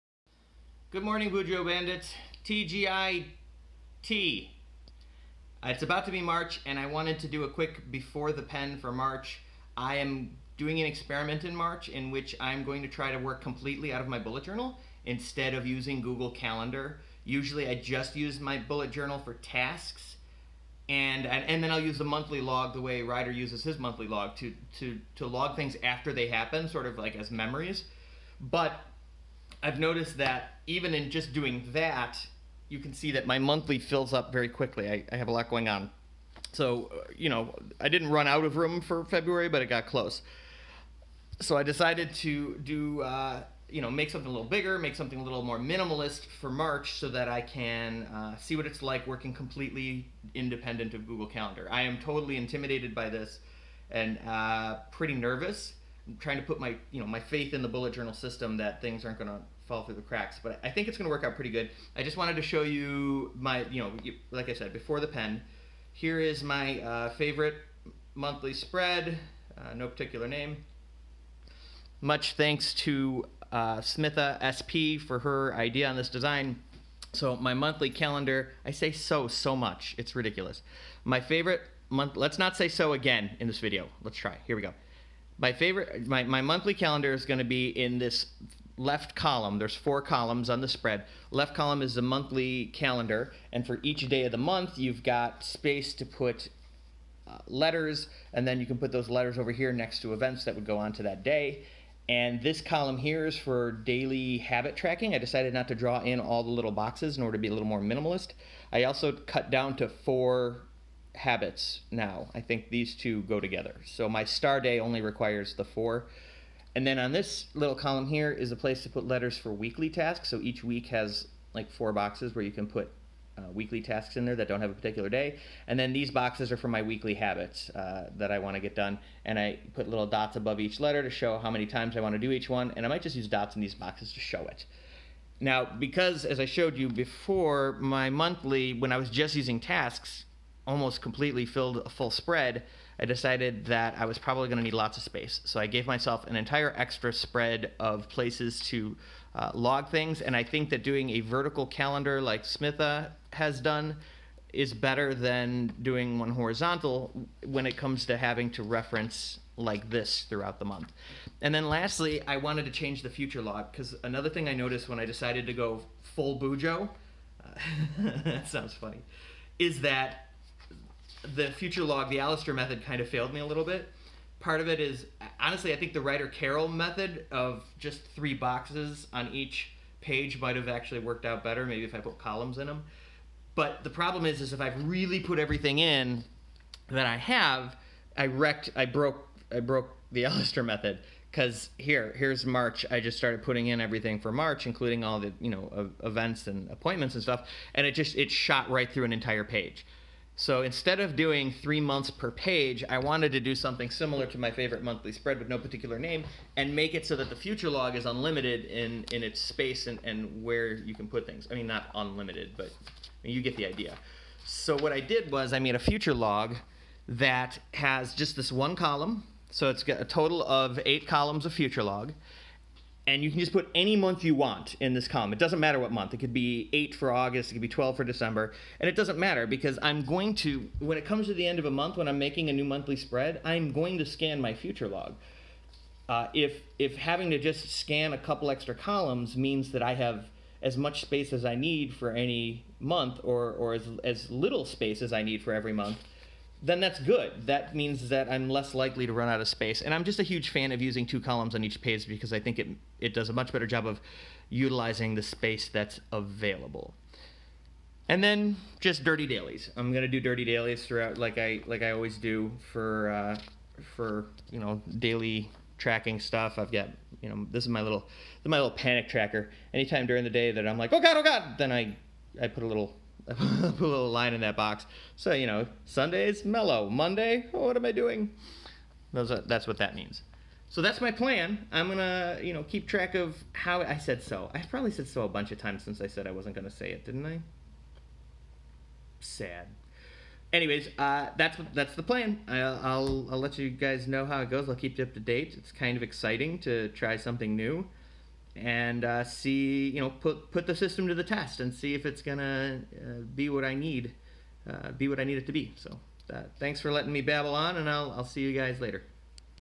Bullet Journal Experiment: Full Analog! No Digital! First use of Frankenlite.
For March, I've decided to try going full BuJo! No Google Calendar. All events, appointments, and tasks in the bullet journal. I'll keep you guys up to date on how things go throughout the month. Here's before the pen.